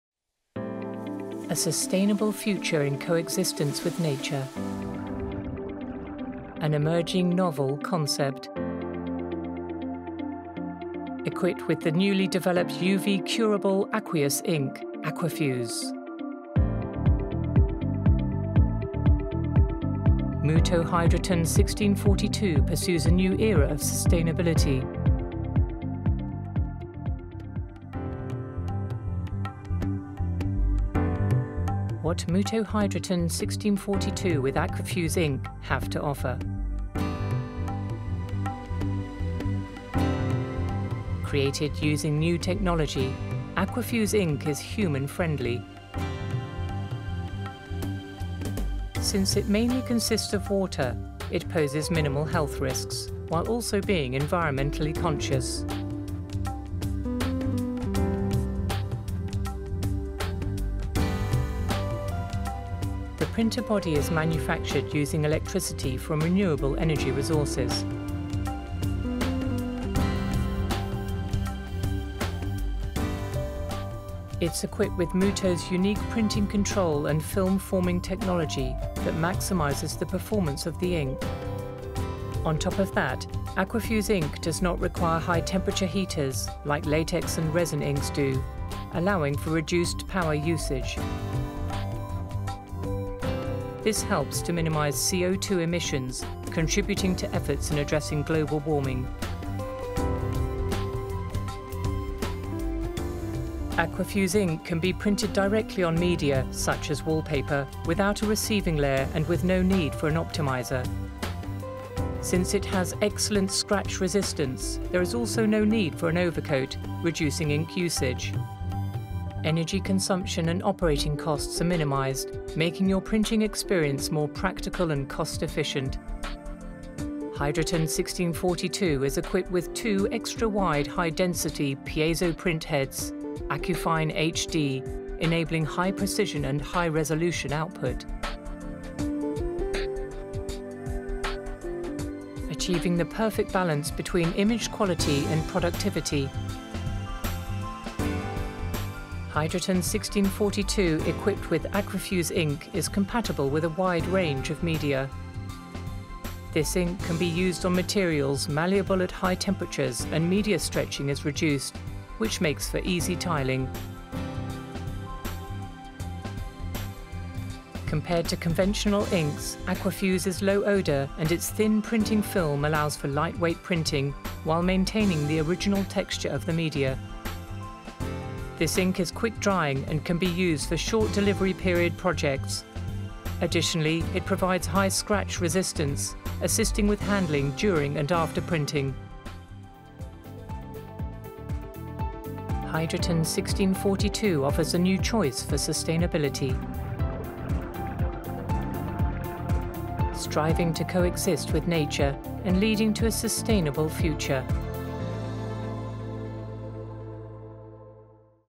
HydrAton 1642 | Roll to Roll AQUAFUZE™ Inkjet Printer | MUTOH (HA-1642)
HydrAton 1642 is a new environmentally-friendly printer, born from the combination of Fujifilm Group's AQUAFUZE™* technology and MUTOH's print control technology. It's equipped with unique printing control and film formation processes that were newly developed to maximise the performance of ink created using AQUAFUZE™ technology.   *AQUAFUZE™ is a registered trademark of Fujifilm Group.

Its highly safe composition, with water as the main ingredient, and its low odour, as well as its capability of printing on different substrates, including using non-penetrating media without the need for primers or optimisers are AQUAFUZE’s™ main characteristics. Moreover, the printed film is thin and offers high scratch resistance without the use of an overcoat, making it suitable for a wide range of applications, including wallpaper, decorations for stores and public facilities, and indoor signs.